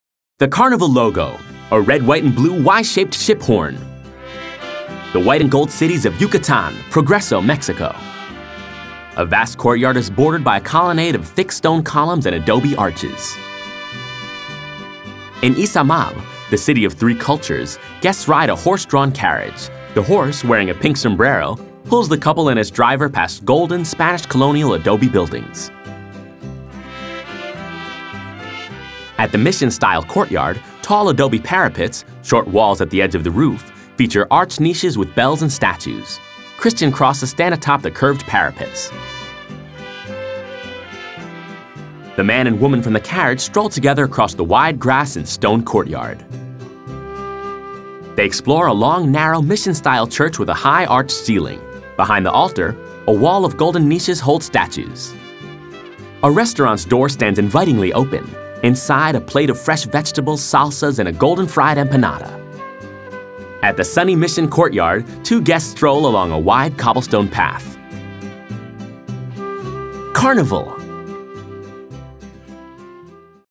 The White and Golden Cities of Yucatan! Yucatan, Mexico | Carnival Cruise Line
Stop at the city of Merida, also known as ""The White City"" and admire the rich Mayan and colonial heritage. Take a scenic tour along the significant boulevard where wealthy Spanish descendants lived and built their mansions. Then make your way over to the ""The Golden City"" known as IzamÃ¡l, founded by the Mayans.

 Hop into a horse-drawn carriage and take a leisurely tour focusing on the Spanish Colonial aspects of this city. Pass by famous sights such as the IzamÃ¡l City Hall which dates back to the 18th century. You will learn a lot on this ancient city tour.

Sound like an excursion you'd like to be a part of?

There's more to see and do in Yucatan, Mexico, check it out